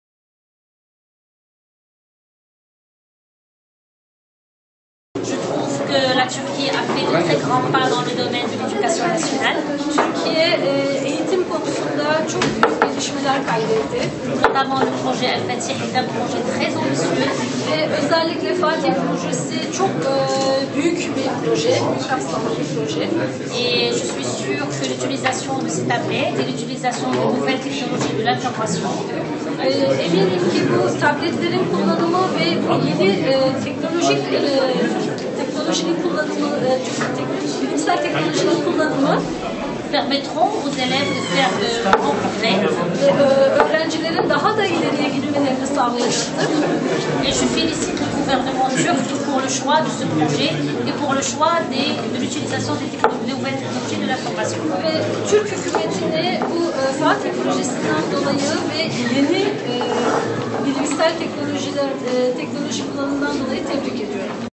كلمة بنخلدون على هامش زيارتها لمشروع مدرسة "الفاتح" التطبيقية بأنقرة
تصريح بنخلدون لـ "الأناضول " التركية على هامش  زيارتها لمشروع مدرسة "الفاتح" التطبيقية بأنقرة، وذلك في اطار زيارة عمل لجمهورية تركيا